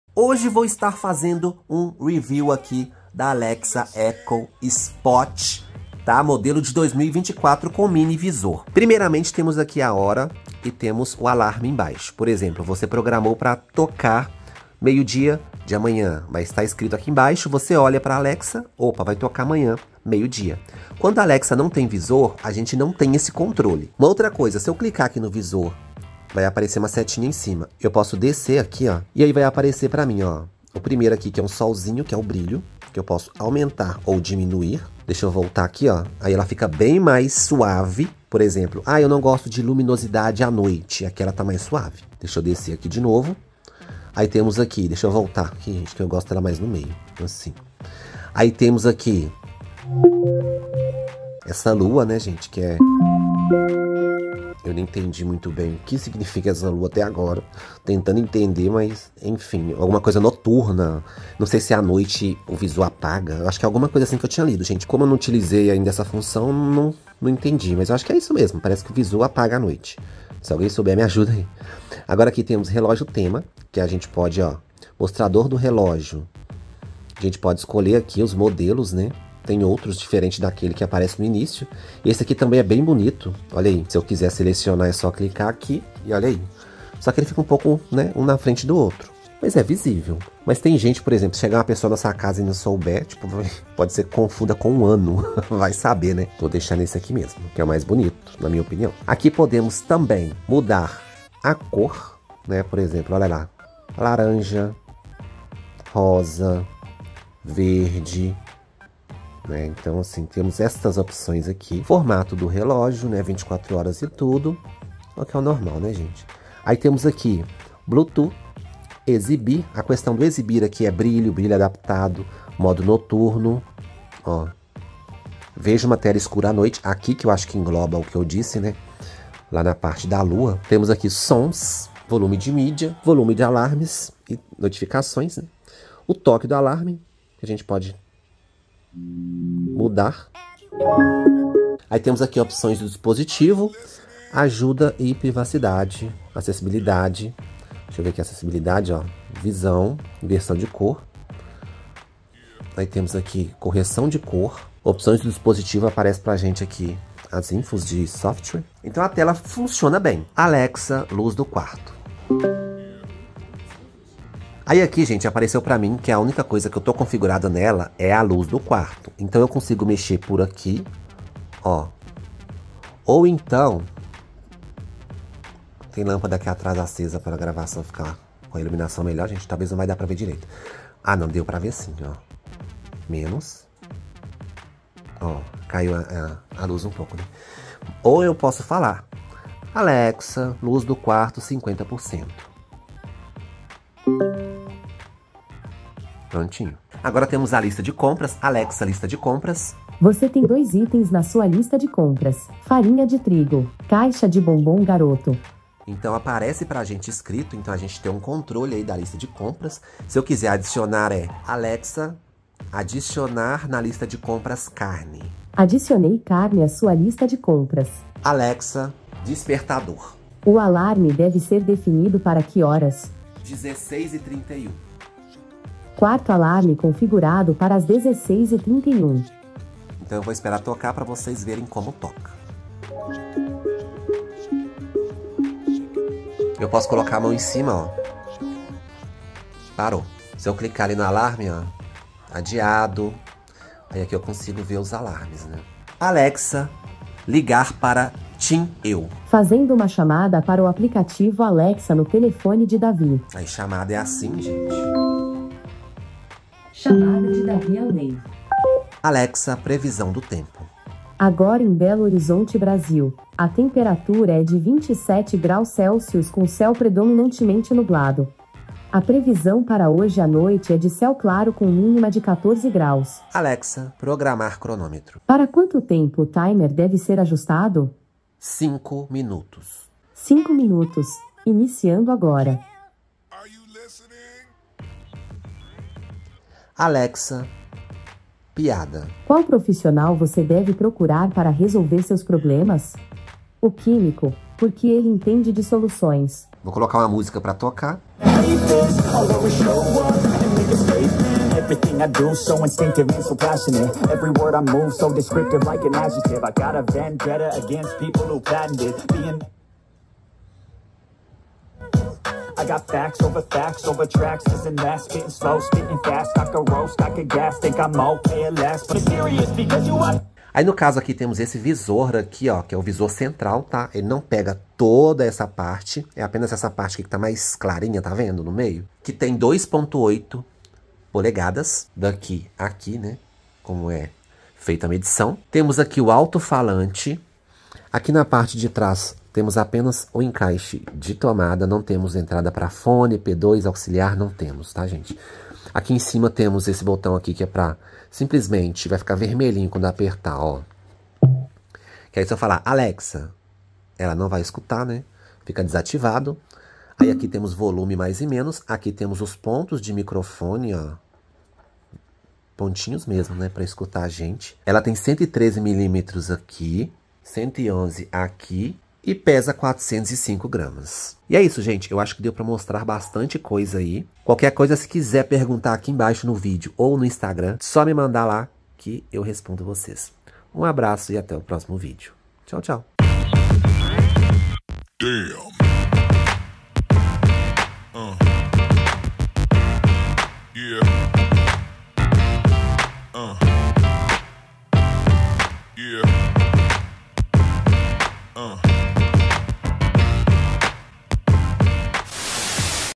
🔊 REVIEW DETALHADO: QUASE TUDO SOBRE O Novo Echo Spot com Alexa (2024) | BRASILEIRO em Português
🟢 No vídeo de hoje, você encontra as seguintes informações:

Nova Alexa Echo Spot

00:00 Como é o visor da nova alexa Echo Spot Touch Screen
00:30 Como alterar o brilho da tela pelo visor
01:15 Como alterar o modelo de relógio
01:35 Mudar a cor do relógio e formato
02:44 Como falar alexa para apagar a luz
03:17 Lista de compras como adicionar itens
03:38 Como criar alarme e como é
04:05 Como fazer uma ligação pela alexa echo spot
04:18 Previsão do tempo e programar cronometro, timer
4:48 Pedir para contar piada
05:00 Detalhes e tocando música, aumentar e diminuir volume
05:22 tamanho do visor e do dispositivo

( Use o campo de busca para encontrar o assunto )